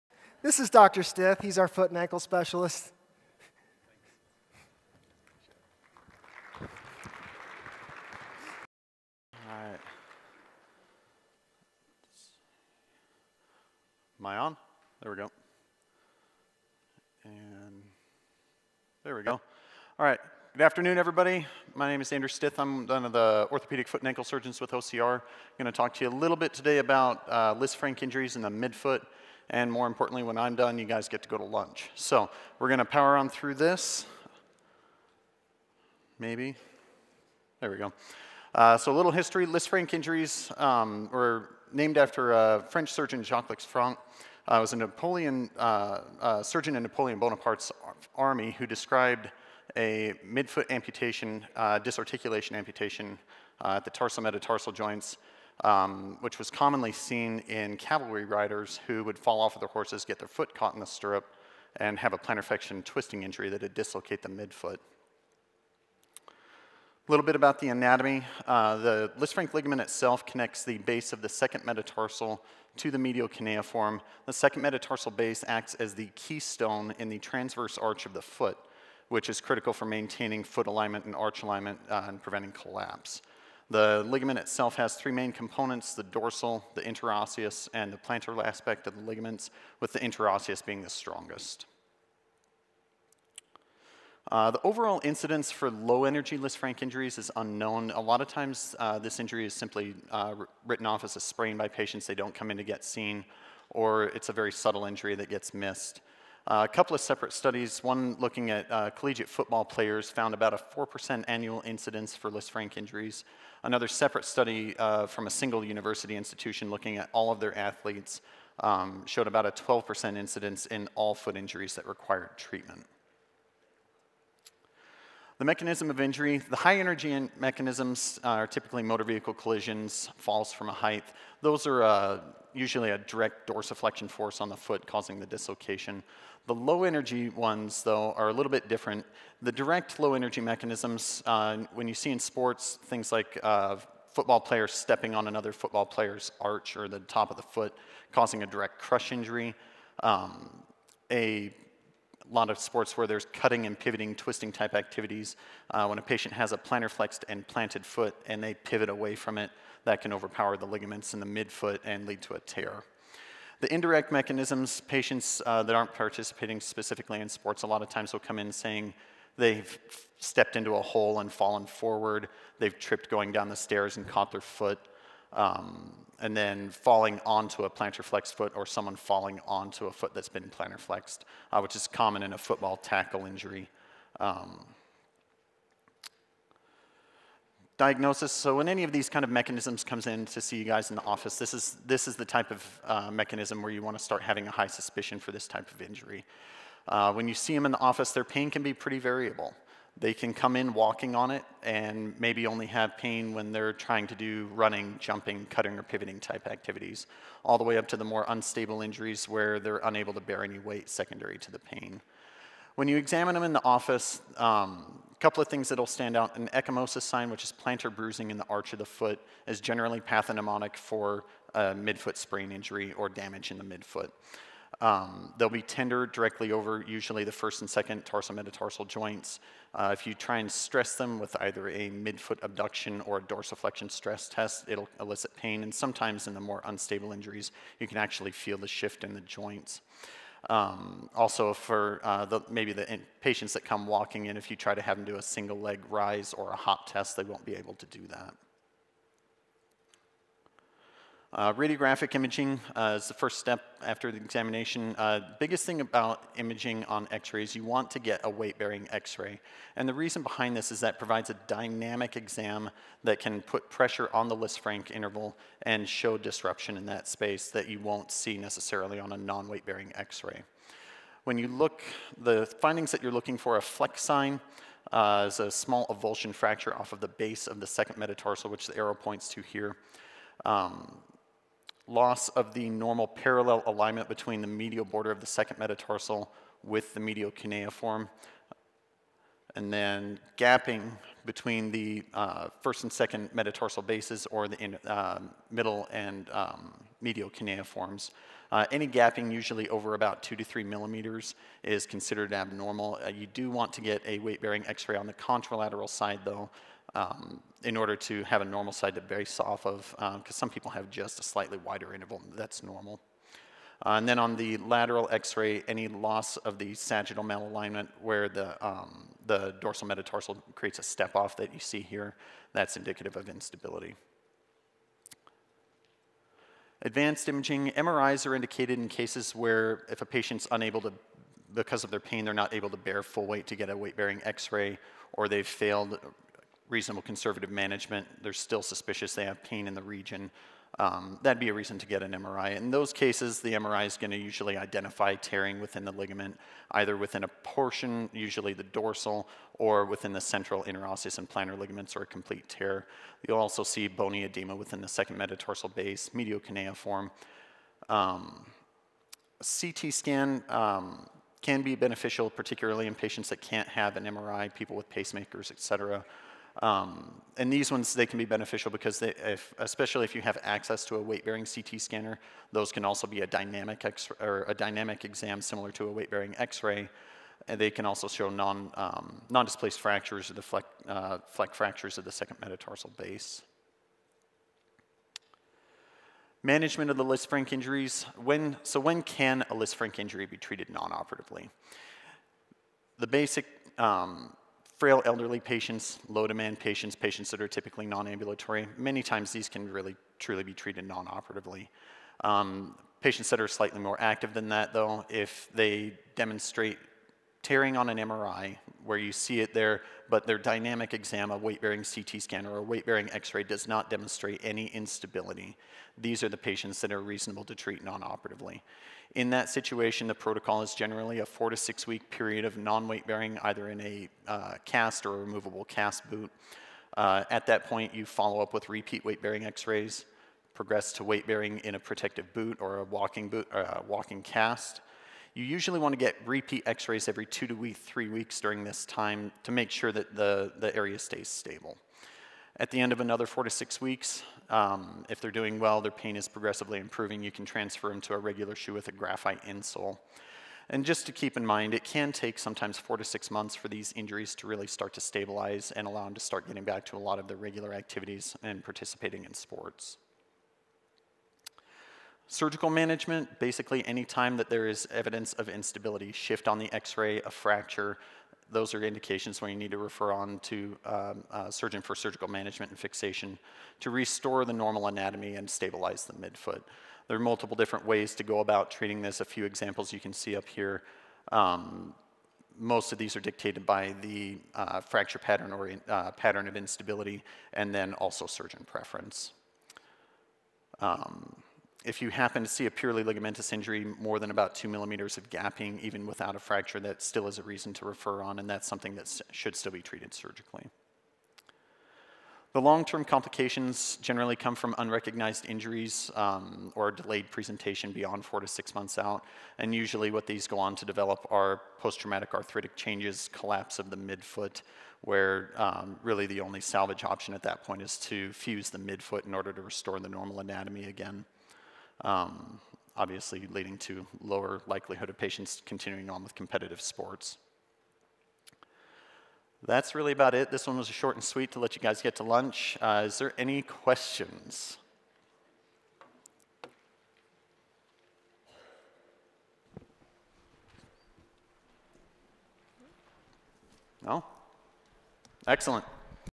Management of Low-Energy Lisfranc Injuries - Orthopaedics Symposium 2022
Management of Low-Energy Lisfranc Injuries
This lecture was given by Andrew Stith, MD, Orthopaedic Surgeon, Foot & Ankle, at the 2022 Orthopaedics Symposium at the Embassy Suites Hotel in Loveland, Colorado.
Watch our website and social media pages for more videos and dates or next years Orthopaedics Symposium.